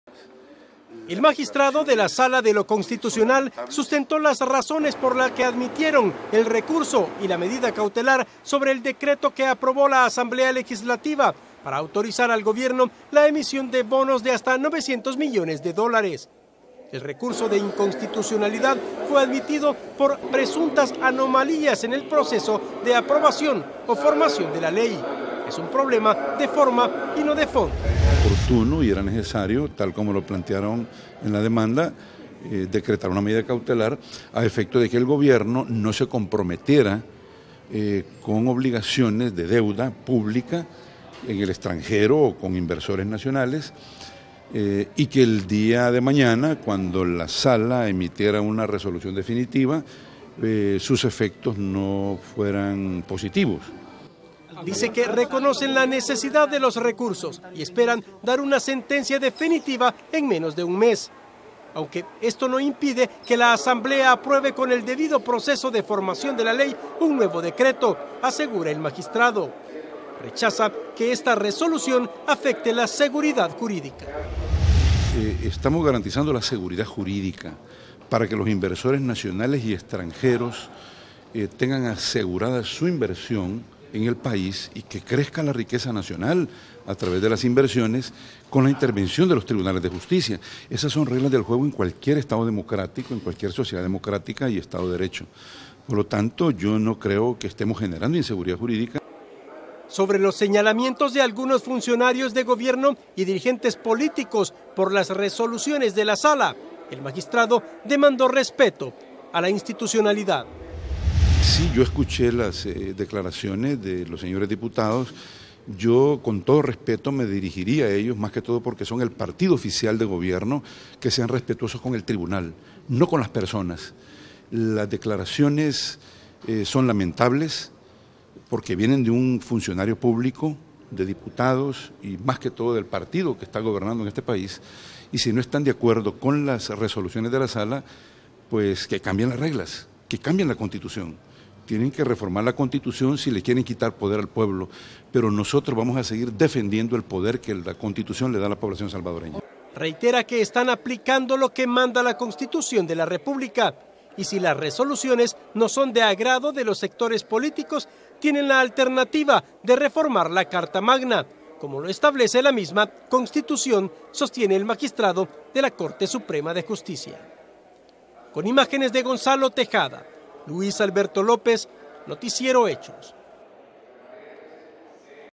DECLARACIONES DE FLORENTIN MELÉNDEZ
Uno de los magistrados de la Sala de lo Constitucional de la Corte Suprema de Justicia defiende las resoluciones que emiten, rechaza que tengan intención de perjudicar al gobierno, además demando más respeto de algunos funcionarios y políticos a la institucionalidad.